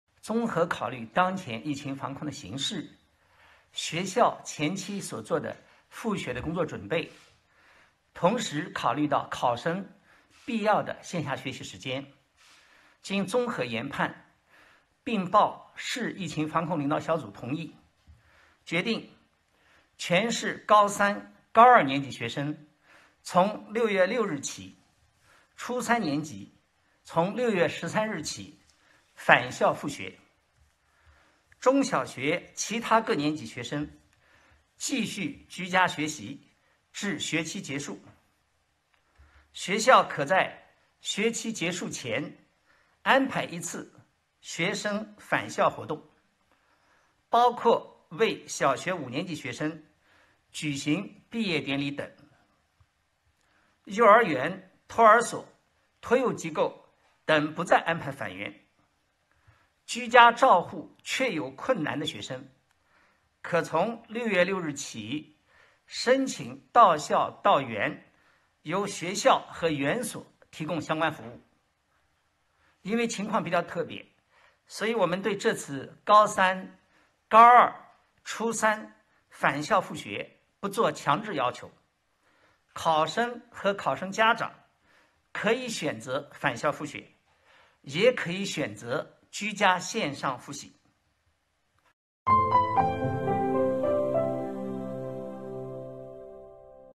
上海：高三、高二年级学生从6月6日起返校复学
在5月26日举行的上海市新冠肺炎疫情防控新闻发布会上，上海市教委副主任杨振峰通报：综合考虑疫情防控形势、学校复学前的准备工作进展、以及考生线下学习的必要时间等因素，经慎重研究并报市疫情防控领导小组同意，决定全市高三、高二年级学生从6月6日，初三年级学生从6月13日起返校复学；中小学其他各年级学生继续居家在线学习至学期结束，学校可在学期结束前安排一次学生返校活动（包括为小学5年级学生举行毕业典礼等）；幼儿园、托儿所、托育机构等不再安排返园。
        居家照护确有困难家庭的学生，可自6月6日起申请到校到园，由学校和园所提供相关服务。因为情况特别，我们对这次高三、高二和初三同学返校复学不做强制要求，考生和考生家长可以选择返校复学，也可以选择居家线上复习。